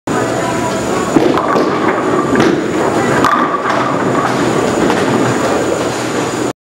Strike #1
Awesome strike - lane 79 at Thunderbowl Lanes - Allen Park MI.